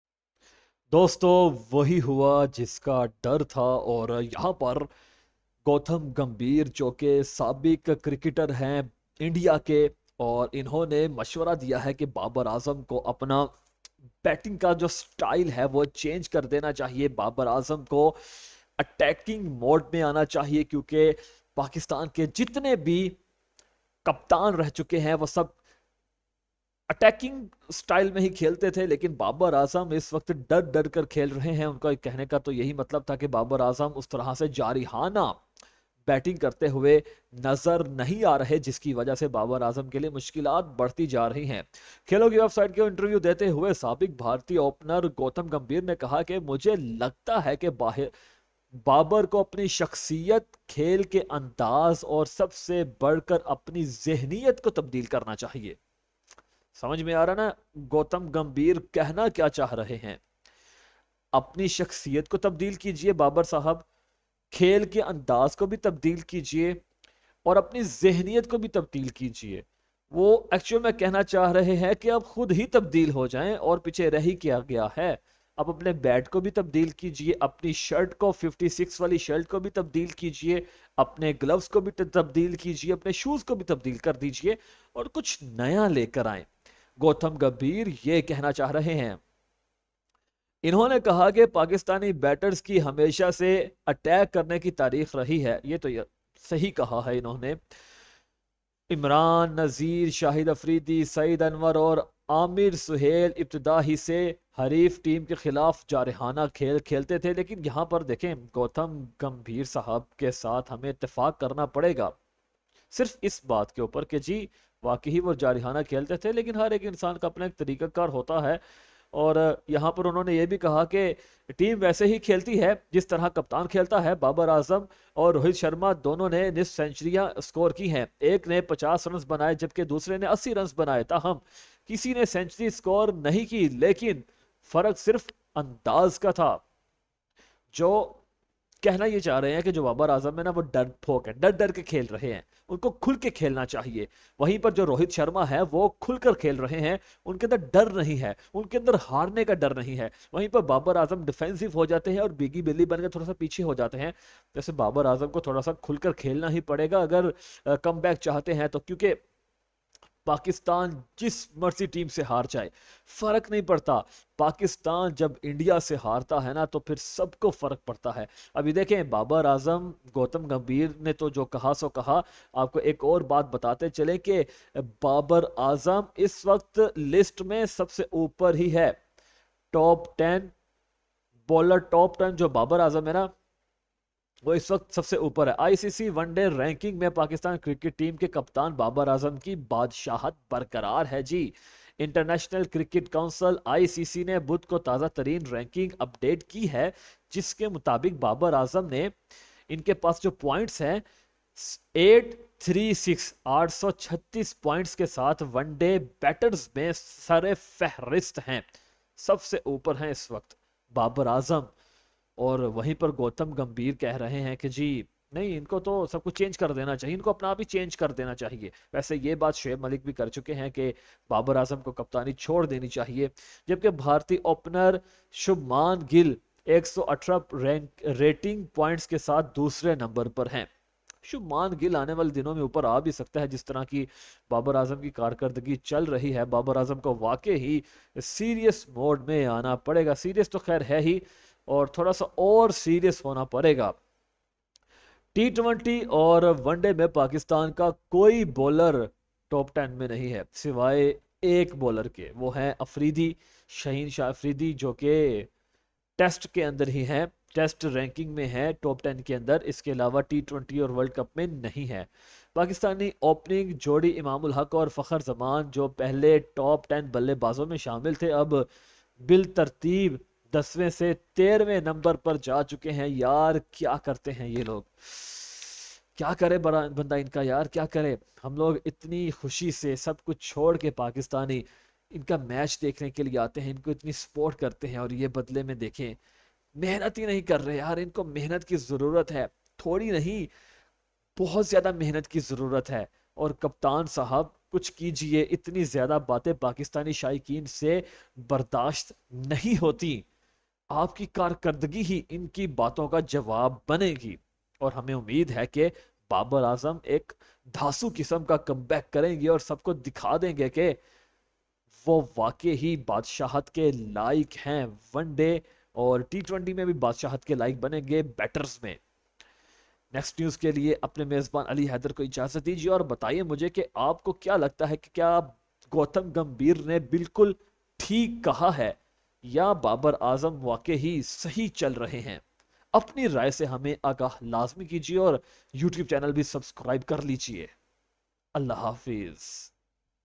Babar Azam Should Change His Batting Style Said Indian Cricketer Gautam Gambhir | Babar Azam is No.1
سابق بھارتی اوپنر گوتم گمبھیر نے کہا کہ ’’مجھے لگتا ہے کہ بابر کو اپنی شخصیت، کھیل کے انداز اور سب سے بڑھ کر اپنی ذہنیت کو بدلنا ہوگا‘‘۔
انہوں نے کہا کہ پاکستانی بیٹرز کی ہمیشہ سے اٹیک کرنے کی تاریخ رہی ہے، عمران نذیر، شاہد آفریدی، سعید انور اور عامر سہیل ابتداء ہی سے حریف ٹیم کے خلاف جارحانہ کھیل کا مظاہرہ کرتے تھے، انہیں نمبر 3 پر انہیں کھلاڑیوں کی طرح ذمہ داری لینا ہوگی.

We Are a Voice of Athletes.